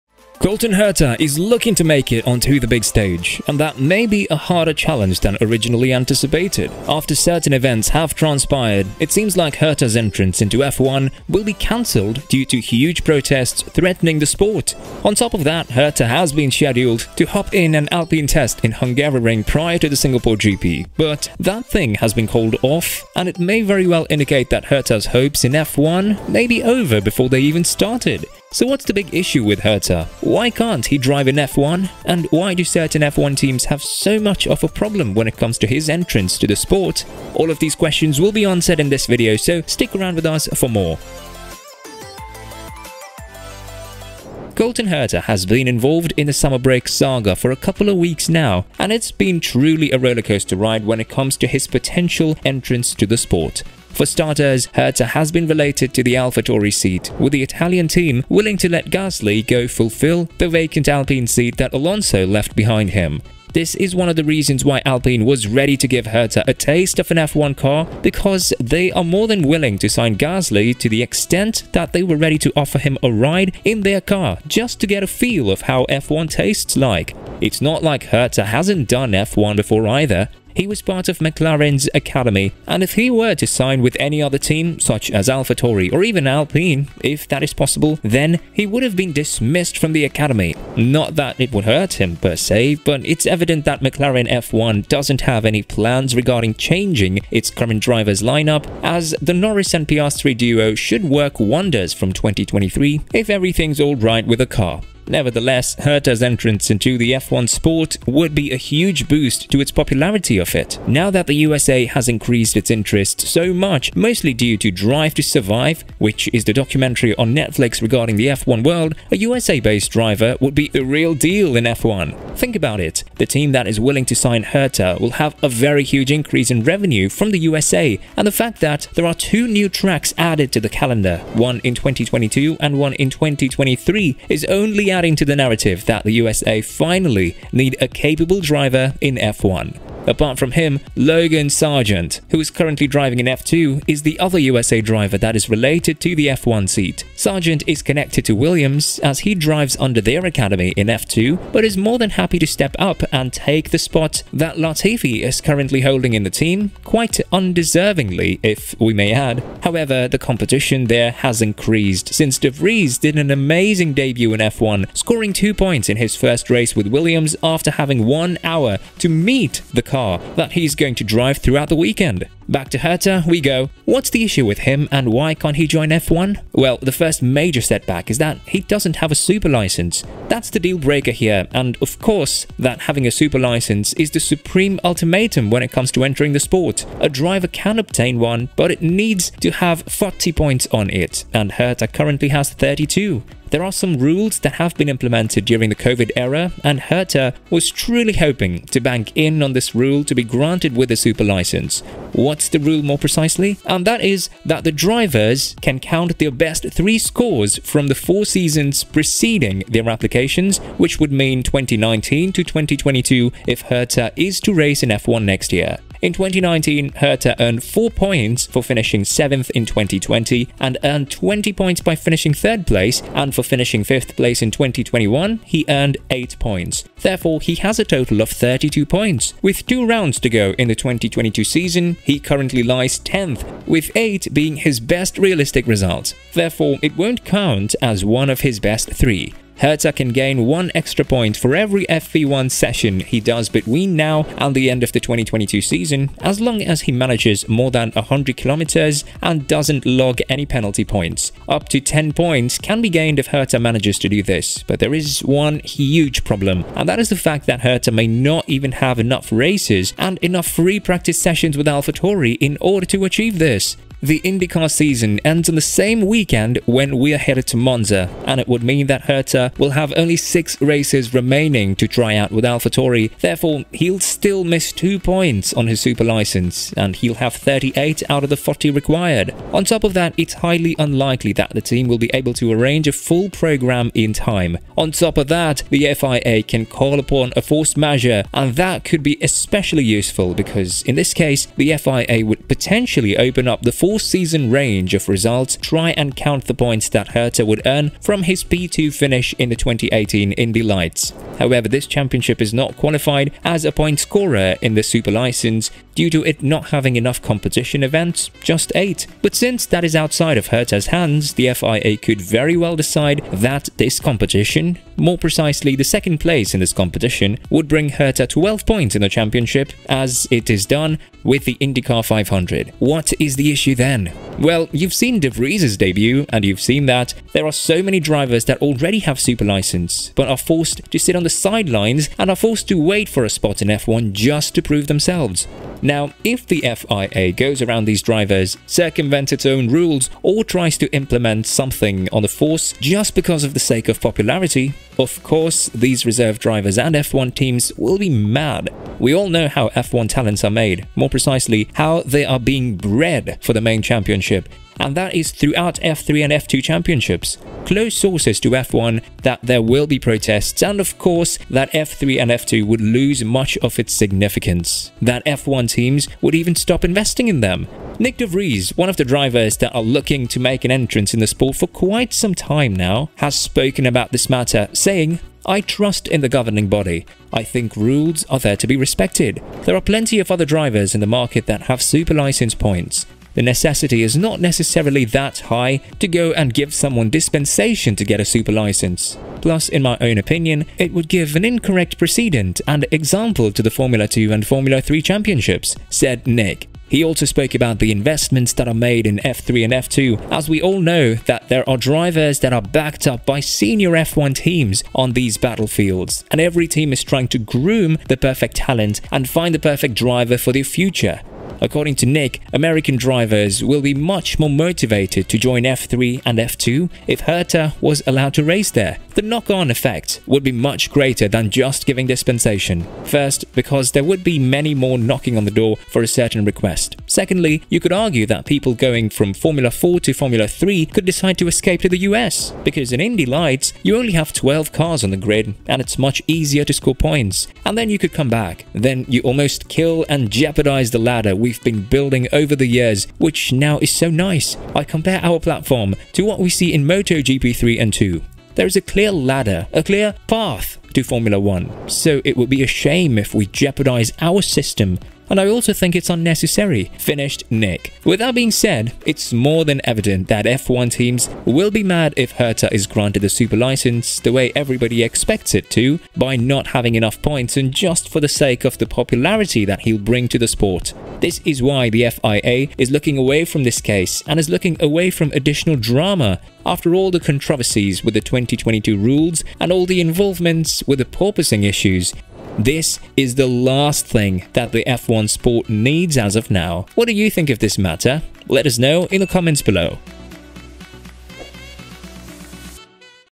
What is the REAL ISSUE With Colton Herta?
You are currently watching: What is the REAL ISSUE With Colton Herta?

Taking into account Colton Herta super license points, we should be confident that there was a good chance that he got his license. However, colton herta f1 future is uncertain, since F1 had something to say.

Colton Herta license is not a sure thing.
May Your Team Win

With Love,

The Undercut Team